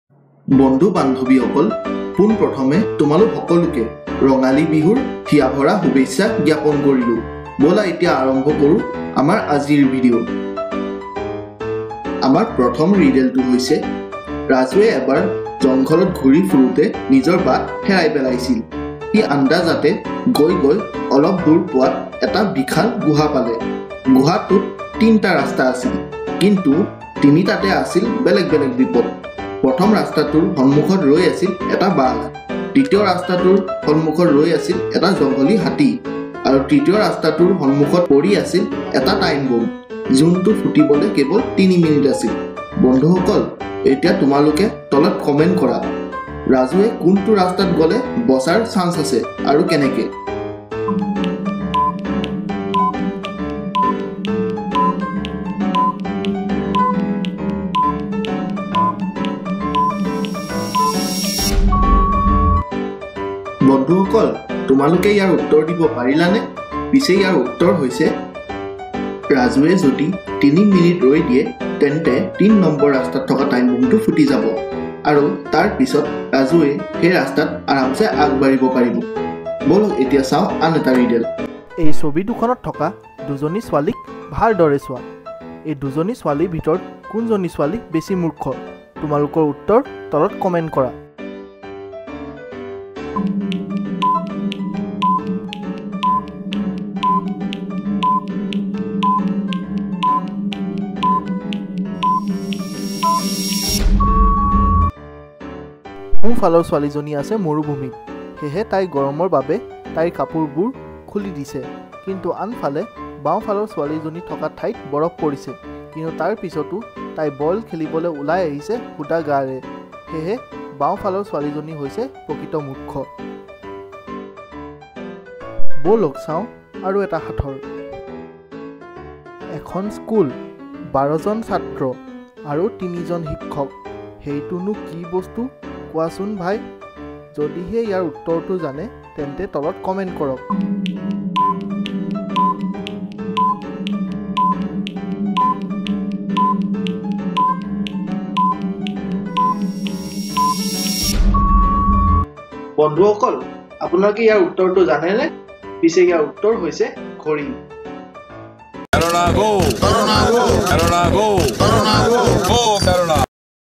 Assamese Riddles (ইয়াত কোন বেছি মূৰ্খ?)
Assamese Riddle and Logic:

Bondhu hokol apunaluk hokolu'key Rongali Bihu aru Axomiya Nava Barxa'r antarik xubesha jashilu.

Aji ami loi ahisu 3ta notun riddle. Aakha korisu jaate apunaluk'e ai video tu sai bhal pai.

Aru jodi apunaluk'or kiba suggestions ase tolot please comment koribo.

জয় আই অসমI (Jai Aai Axom!)